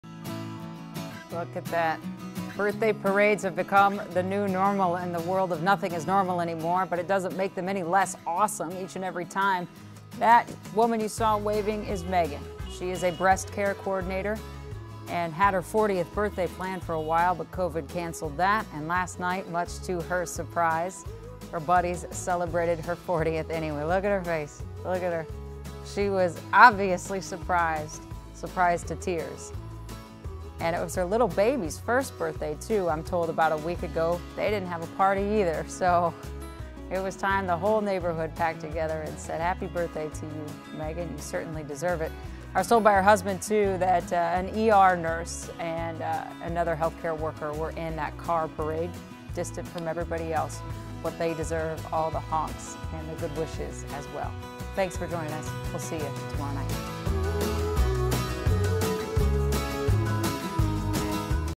Birthday parade in MPLS
Megan, a breast cancer coordinator, had her birthday plans canceled because of the coronavirus. Her friends, however, weren't about to let her not celebrate turning 40. Megan's friends came together for a birthday parade to help make the day a little more special.